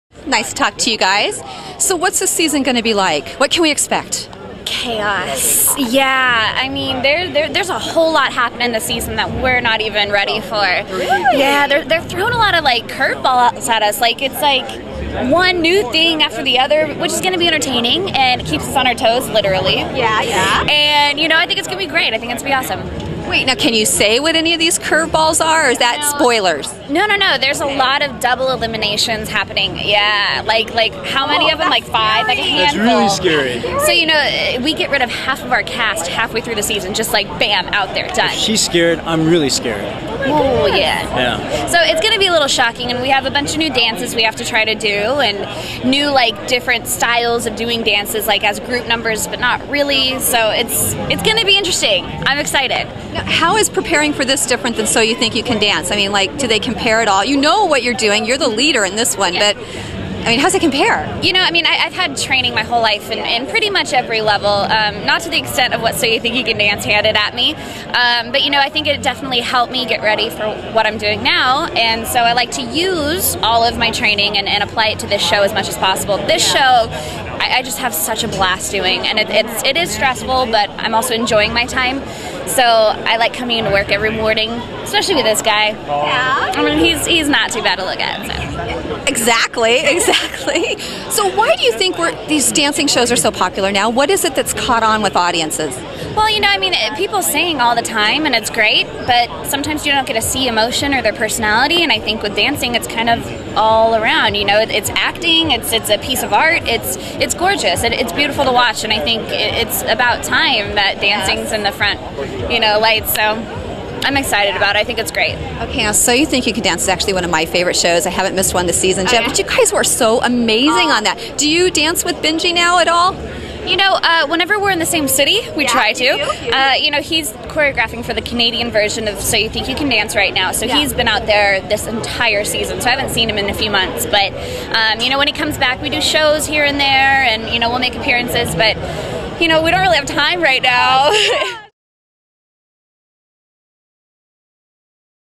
Lacey Schwimmer and Mark Dacascos Interview
Interview with Lacey Schwimmer and Mark Dacascos discuss Dancing With the Stars, routines, and practicing.  The interview took place at the Whiteout premiere in Hollywood, CA in September 2009.